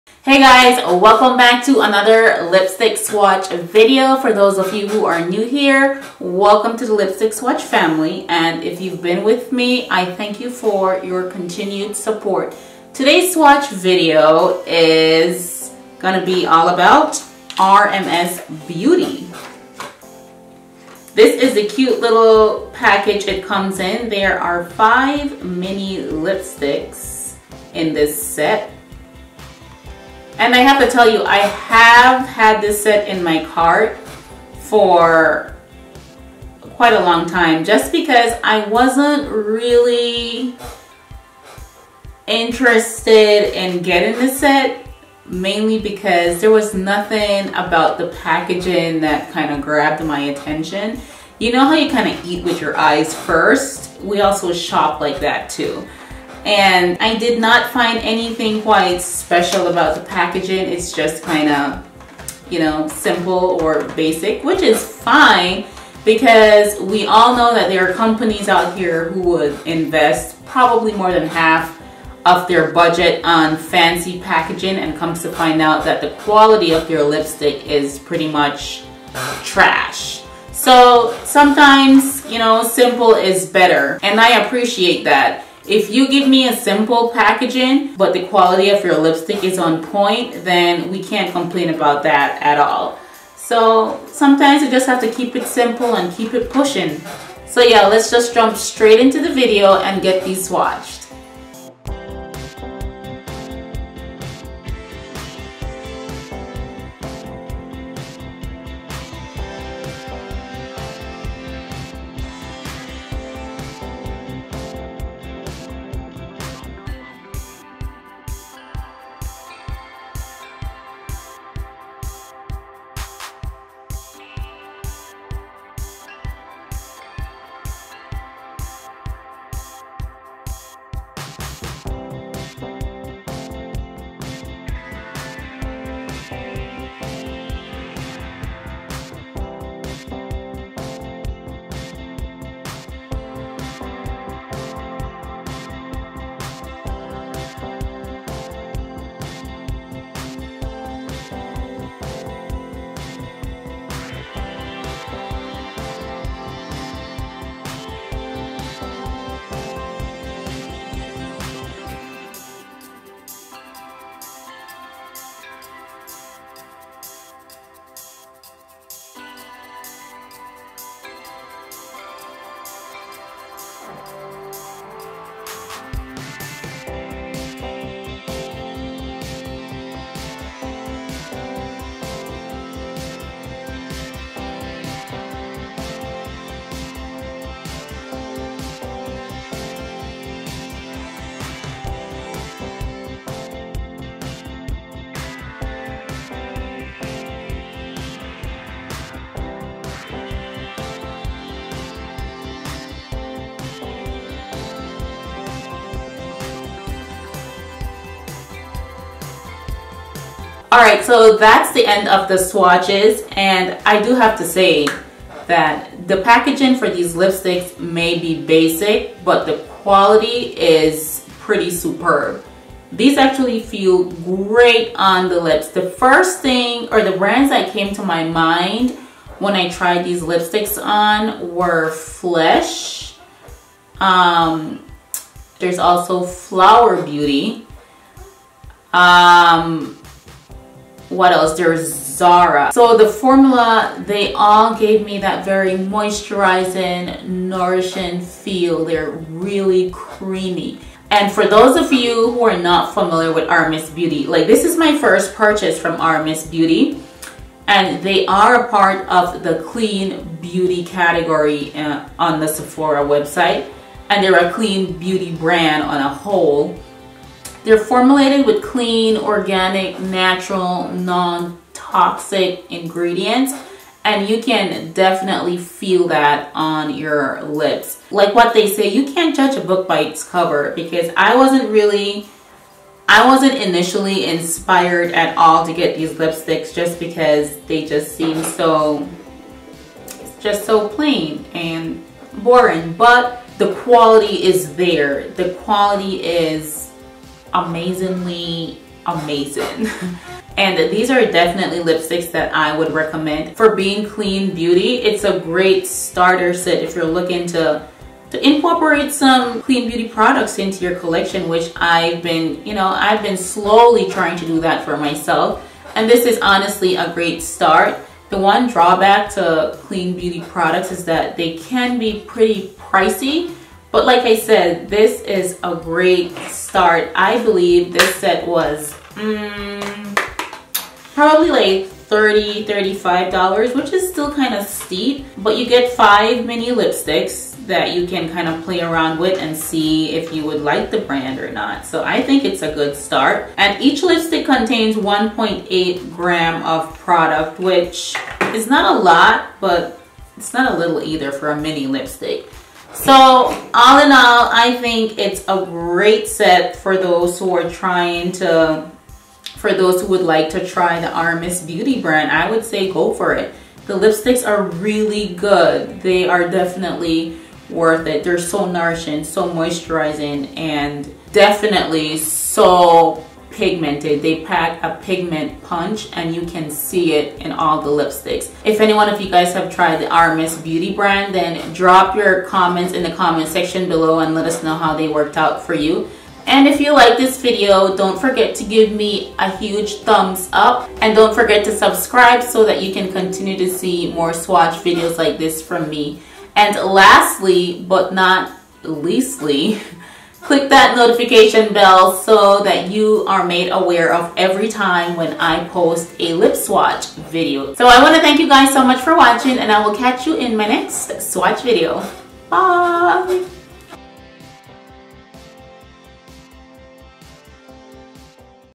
RMS Beauty Wild With Desire Lipstick Swatches | PuckerUpBabe
Hey guys so I finally got to try Flower Beauty Lipsticks. I was quite impressed.

Let me know what you guys think in the comments. What other lipstick swatches would you like to see?